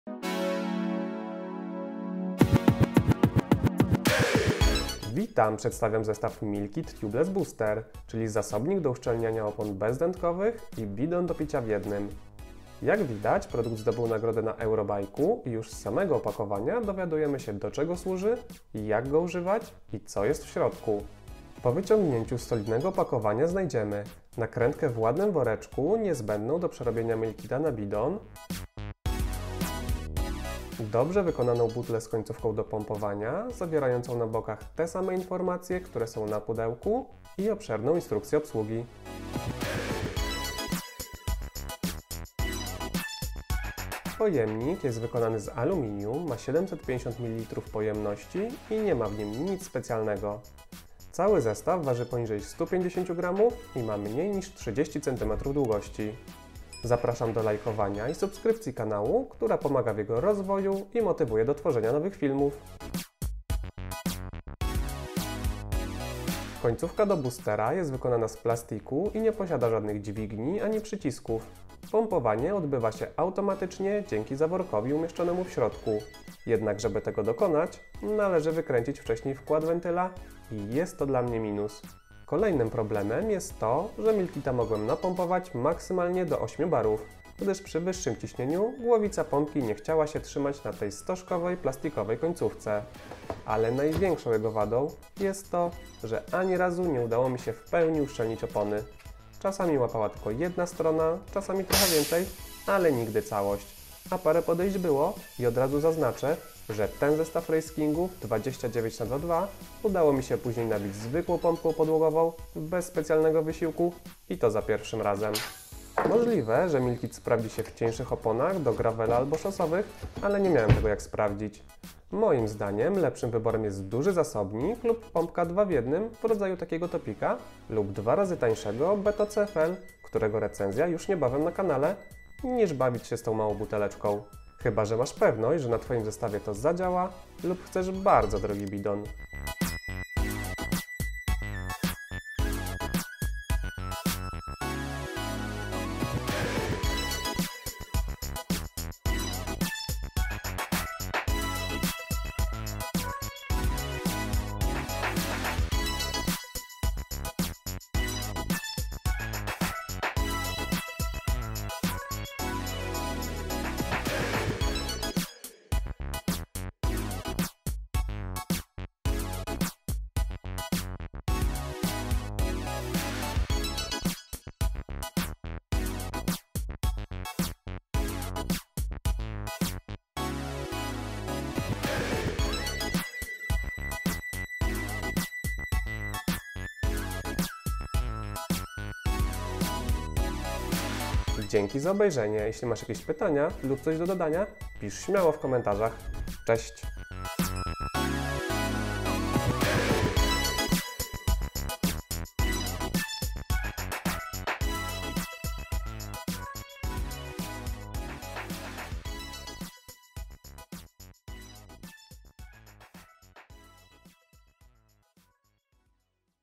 MILKIT TUBELESS BOOSTER // Czy Poradzi Sobie Z Oponami Bezdętkowymi MTB ?
Przedstawiam zestaw Milkit Tubeless Booster, czyli zasobnik do uszczelniania opon bezdętkowych i bidon do picia w jednym.

👉 Z A P R A S Z A M D O O B E J R Z E N I A